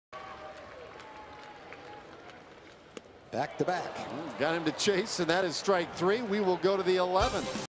TEX@CLE: Claudio strikes out Martinez swinging
Alex Claudio strikes out Michael Martinez on three pitches to end the bottom of the 10th inning